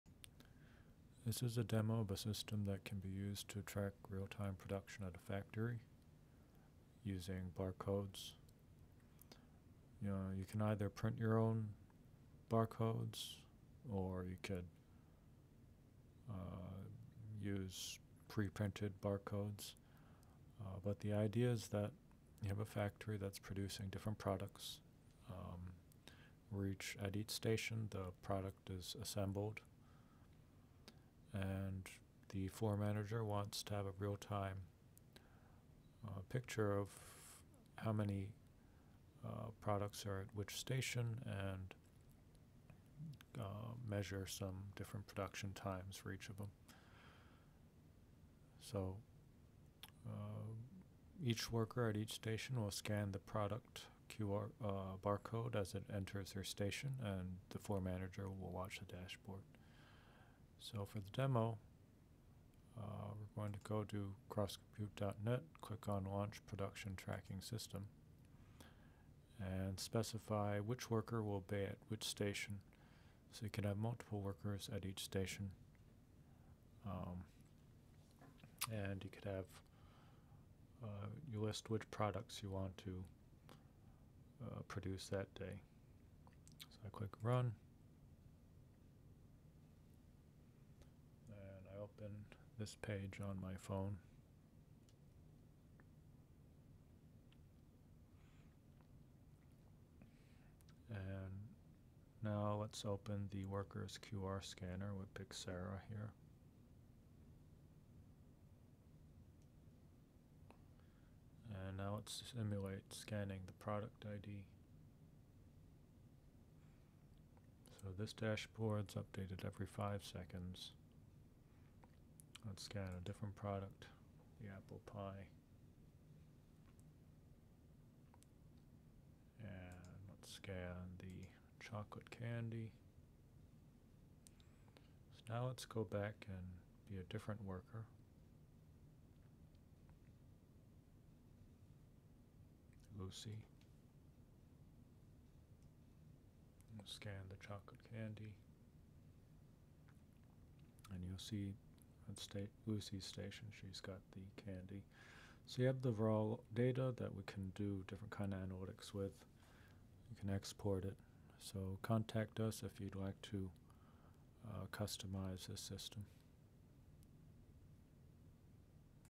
Launch Your Own Production Tracking System for Your Factory
Here we present a demo of a system for tracking production times at a factory manufacturing physical products such as utility poles. A barcode is affixed to a product when it enters the manufacturing process. Workers scan the product barcode upon receiving the product at their station.

Thanks to Raymundo, Liliana and Guillermo Durán of Lumiservicios, S.A. de C.V.
Thanks to Kashfi Fahim.